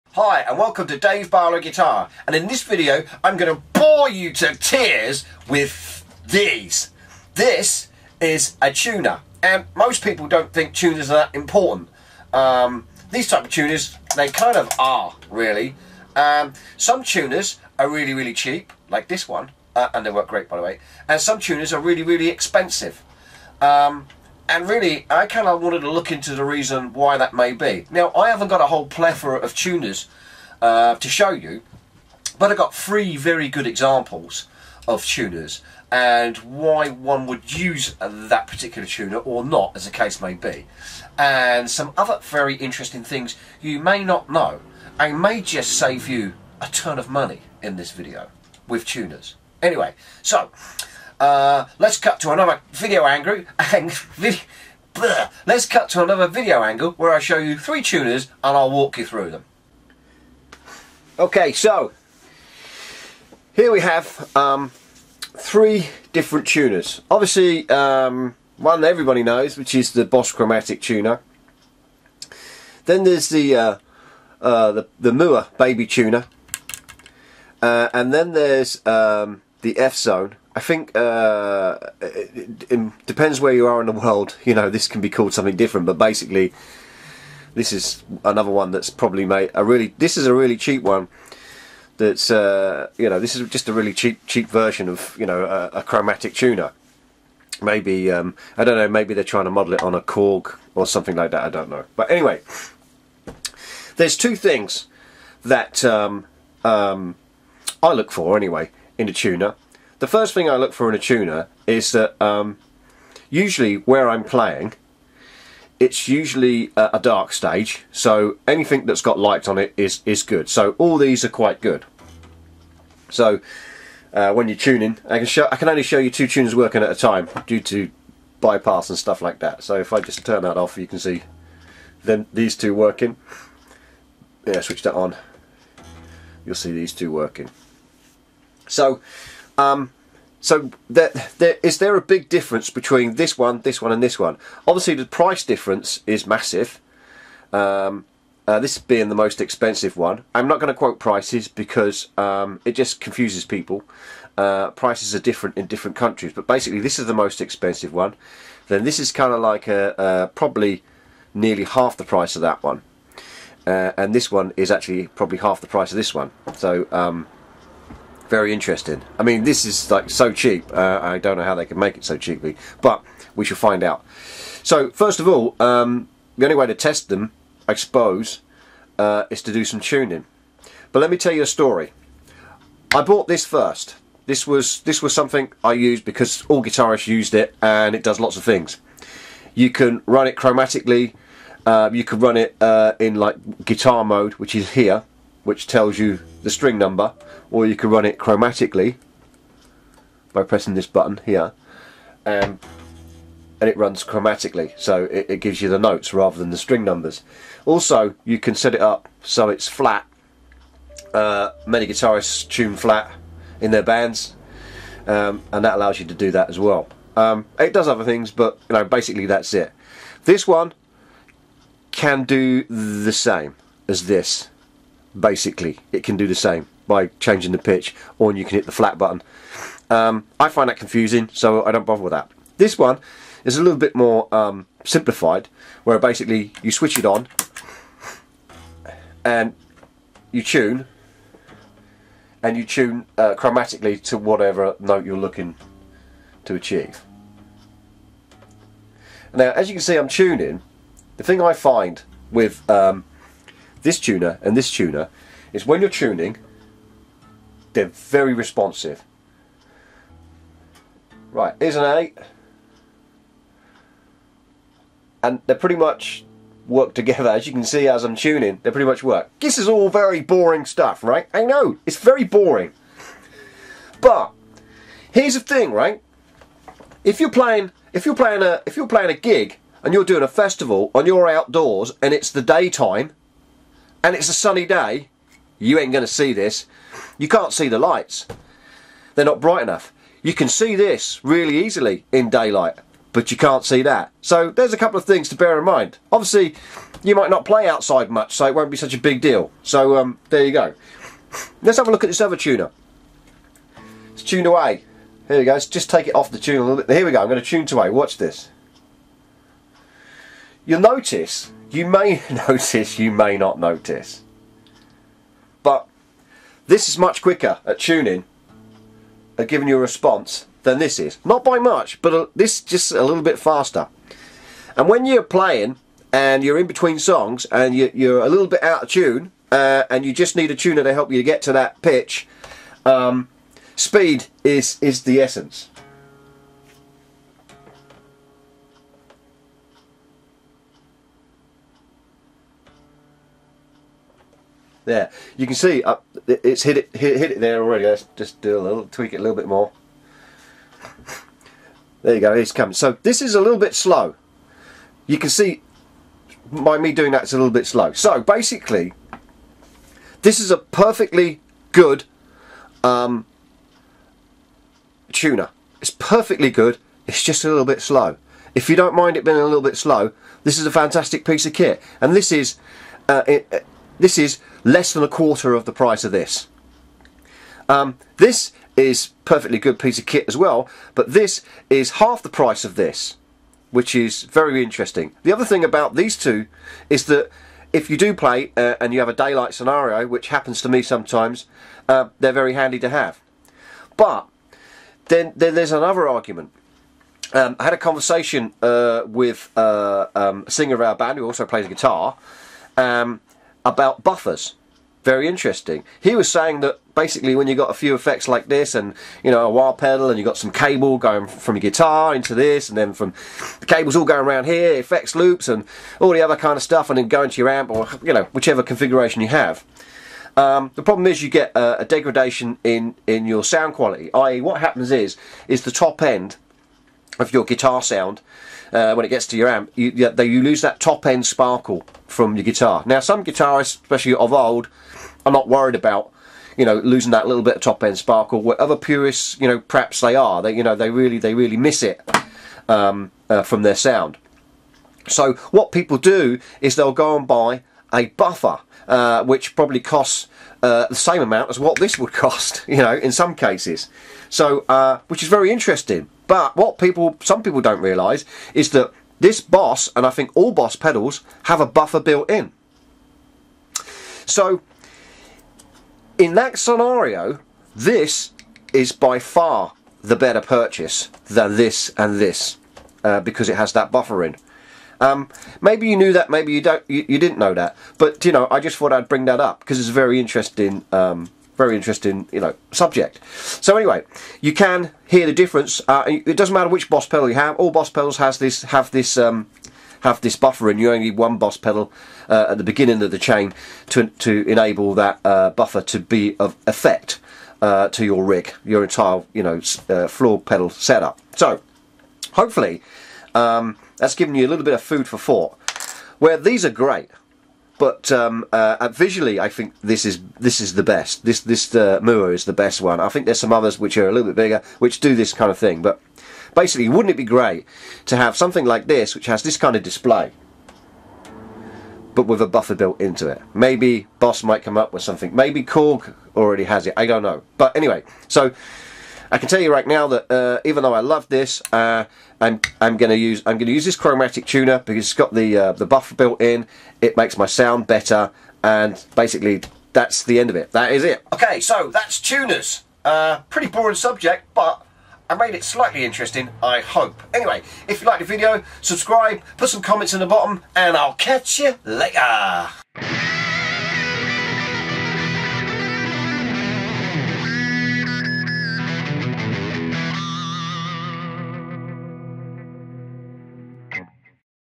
Guitar Tuners and the Buffer Question
Three guitar tuners to check out here, Boss Chromatic Tuner, The Mooer Chromatic Tuner and the F Zone Chromatic Tuner, which one is the best and which one would you use.

Also I explain the extra benefits of the Boss Tuner and it may surprise you.

Have you downloaded my app yet?